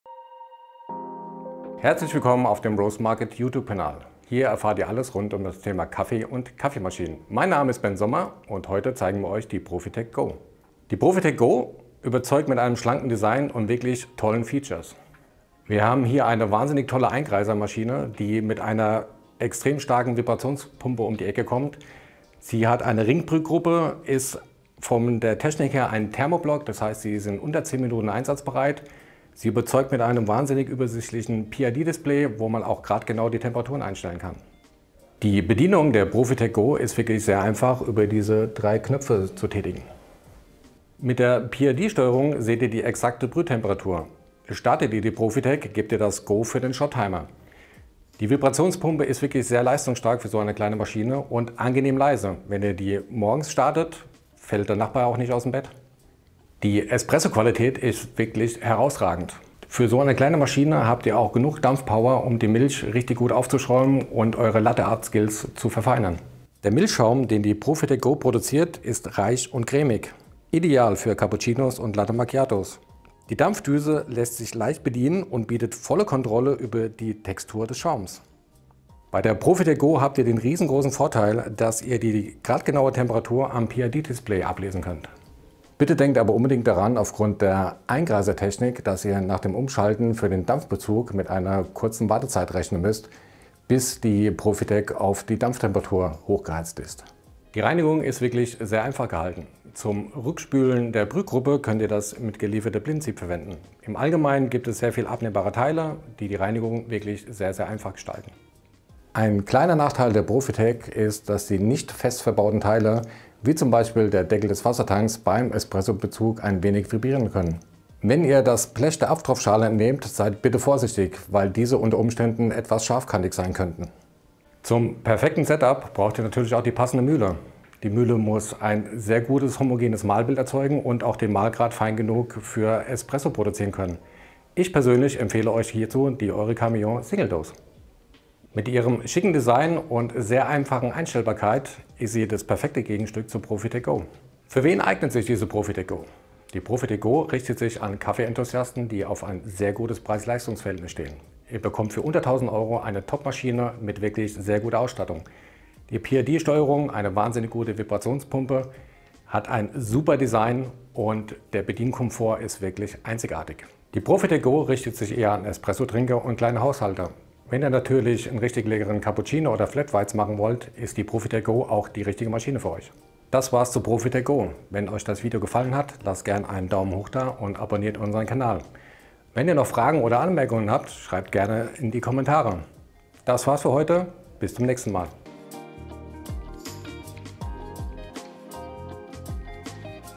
Profitec-Go Einkreiser-Siebträgermaschine | roastmarket
Die Profitec GO ist eine stylische und praktische Siebträgermaschine, die durch ihre Optik und Funktion in jede Küche passt. Wir haben sie zusammen mit Kaffee-Experte Ben Sommer etwas genauer unter die Lupe genommen, um sie mit anderen kleinen Espressomaschinen in der Preisklasse zu vergleichen. Die einfache Bedienung macht sie auch für Anfänger geeignet. Die Profitec Siebträgermaschine hat ein Einkreiser-System mit Ringbrühgruppe, d.h. der Kessel sitzt direkt über dem Brühkopf. Daneben punktet der kleine Einkreiser mit PID-Regulierung, Shot-Timer, Schnellheizsystem (5-7 Minuten) und starker Dampflanze. Was kann die Profitec GO und was macht sie im Vergleich besonders? All das seht Ihr in unserem neuen Video.

► Beste Kaffees, hochwertiges Zubehör und kompetente Beratung: Hier geht's zu unserem Shop